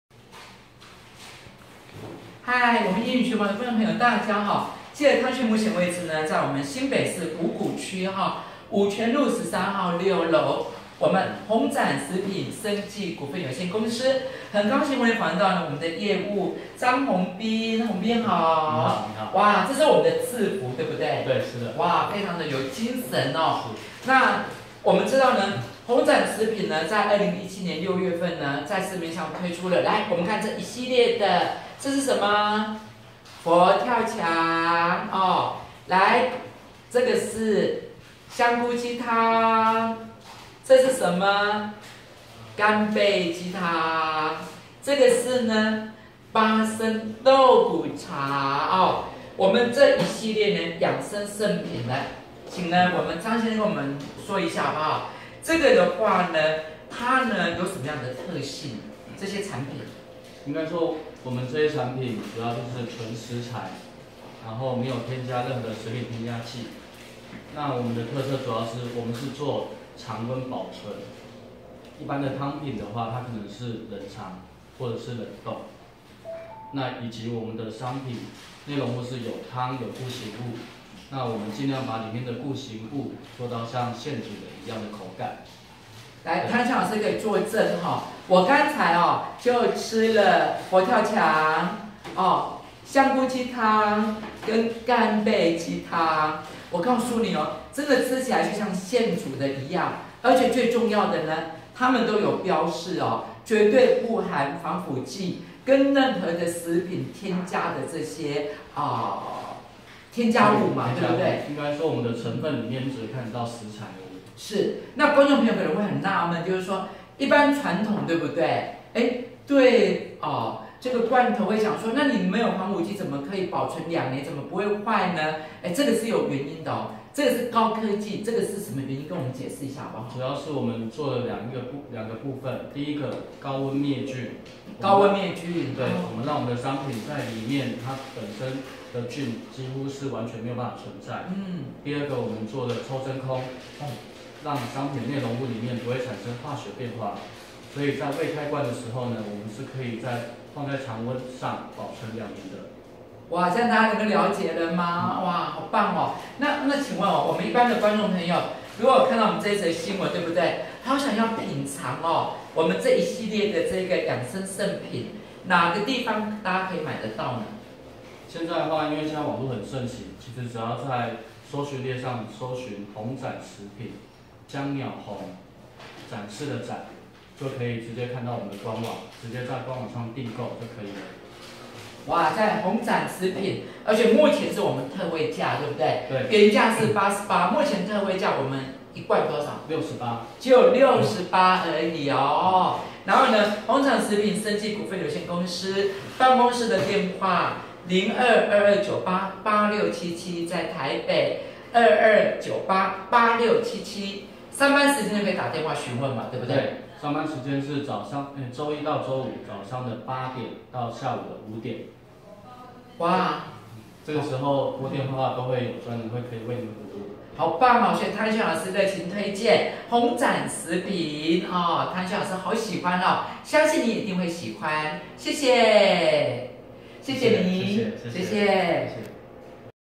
鴻展食品 生技股份有限公司 鮮盒子養生聖品市場評價好，湯尼陳超喜歡！
位在新北市五股區五權路13號6樓，鴻展食品生技股份有限公司是目前台灣唯一製造鮮盒子養生聖品的公司。有佛跳牆干貝雞湯巴生肉骨茶，香菇雞湯 定價每個鮮盒子88元，目前特價68元 加熱就可馬上使用 非常的便利 採用高溫滅菌，抽真空，保存期限兩年 絕對是自用贈禮最佳的 選擇，歡迎上班時間電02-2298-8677 洽詢訂購。